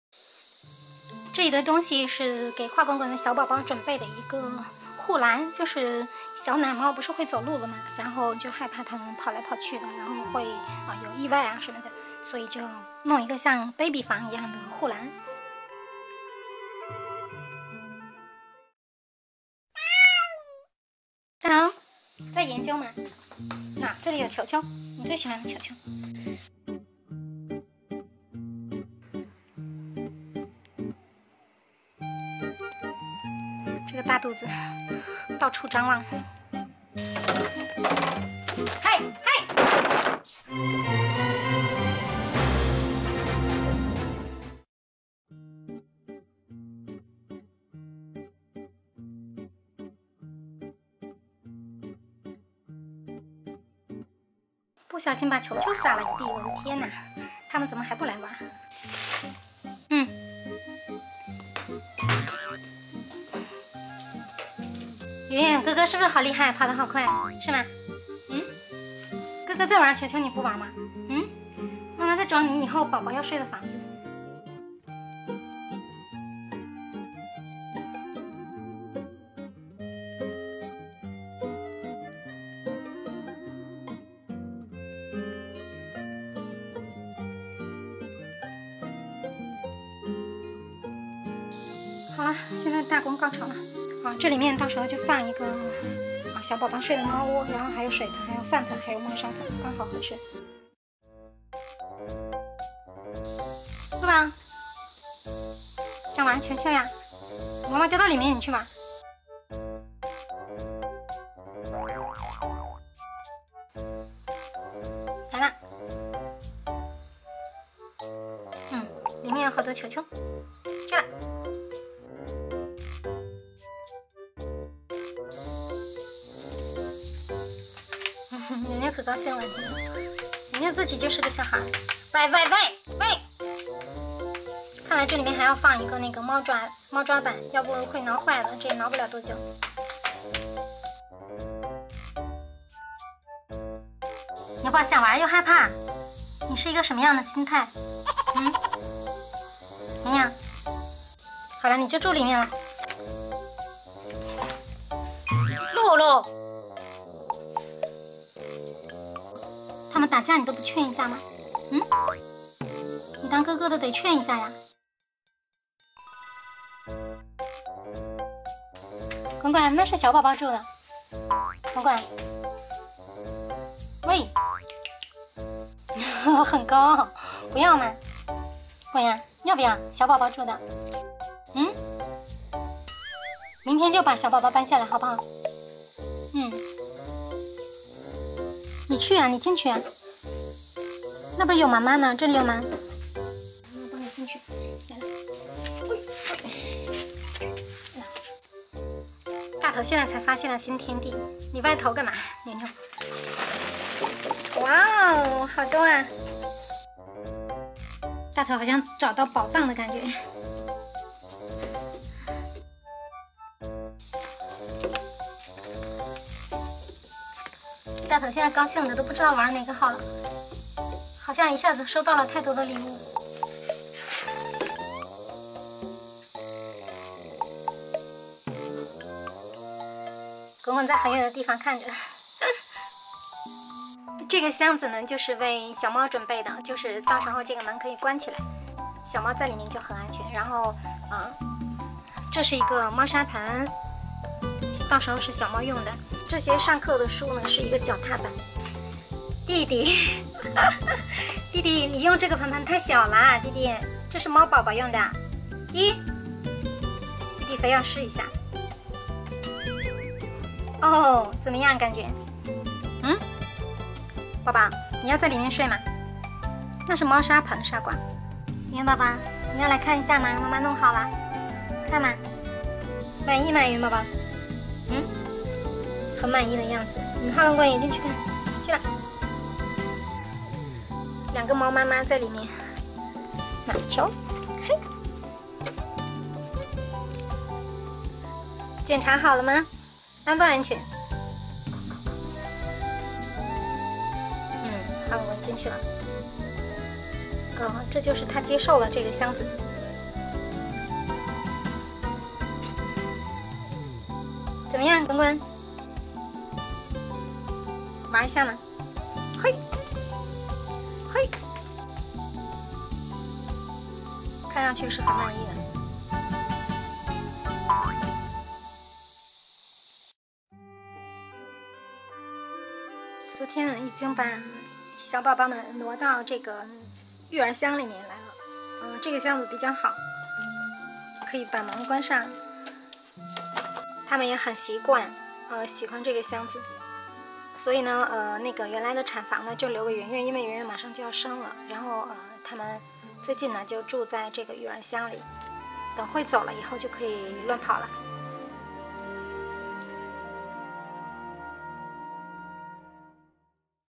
女主人为小奶猫搭建了豪华育婴房，大猫们表示很不服：我们也要玩！ ｜李喜猫
小猫咪们就快学走路了，女主人为防止意外专门搭建了萌哒哒的育婴学步室，猫妈妈看上去非常满意！

分享1位铲屎官和13只猫咪及1只狗的幸福生活！
周日晚上七点半在线直播，
影片中的猫咪跟现实中的猫咪有什么不一样的表现，等你来发掘！ ❤❤❤